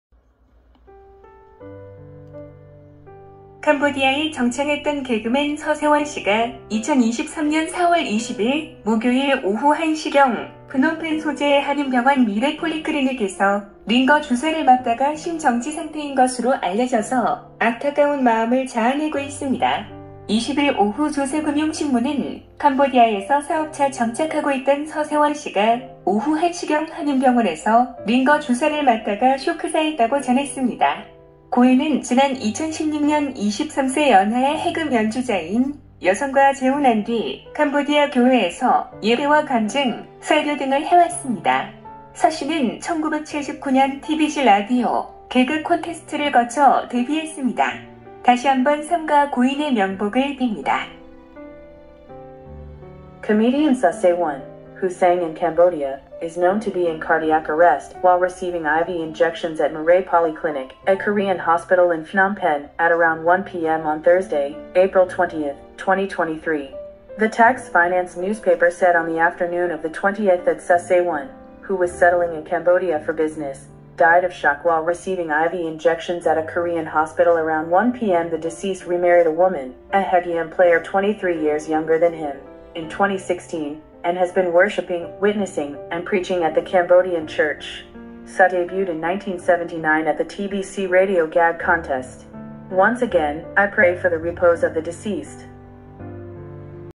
서세원씨 별세 삼가 고인의 명복을 빕니다
캄보디아에 정창했던 개그맨 서세원씨가 2023년 4월 20일 목요일 오후 1시경 프놈펜 소재의 한인병원 미래폴리크리닉에서 링거 주사를 맞다가 심정지 상태인 것으로 알려져서 아타까운 마음을 자아내고 있습니다. 20일 오후 조세금융신문은 캄보디아에서 사업차 정착하고 있던 서세원씨가 오후 1시경 한인병원에서 링거 주사를 맞다가 쇼크사했다고 전했습니다. 고인은 지난 2016년 23세 연하의 해금 연주자인 여성과 재혼한 뒤 캄보디아 교회에서 예배와 간증, 설교등을 해왔습니다. 서씨는 1979년 TBC라디오 개그 콘테스트를 거처 데뷔했습니다. 다시 한번 삼가 고인의 명복을 빕니다.

Comedian Seo Se-won, who sang in Cambodia, is known to be in cardiac arrest while receiving IV injections at Mirae Polyclinic, a Korean hospital in Phnom Penh, at around 1 p.m. on Thursday, April 20, 2023. The Tax Finance Newspaper said on the afternoon of the 20th that Seo Se-won, who was settling in Cambodia for business, died of shock while receiving IV injections at a Korean hospital around 1 p.m. The deceased remarried a woman, a haegeum player 23 years younger than him, in 2016, and has been worshiping, witnessing, and preaching at the Cambodian church. Seo debuted in 1979 at the TBC Radio Gag Contest. Once again, I pray for the repose of the deceased.